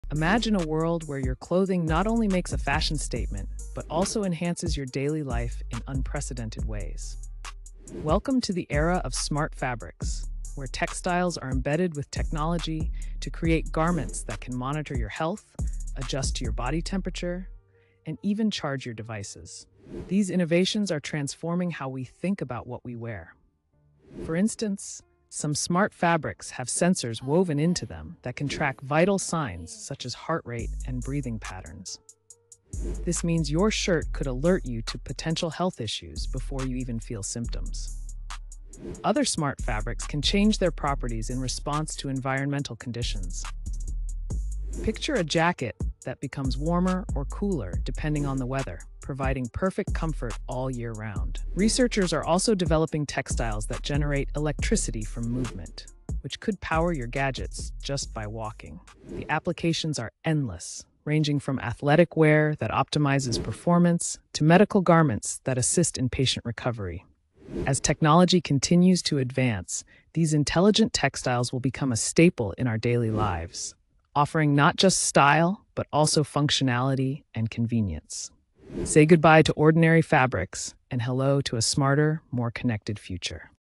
Smart Fabrics: The Future of Everyday Clothing
Discover the incredible world of smart fabrics! From health monitoring to climate control, see how our clothes are getting smarter every day.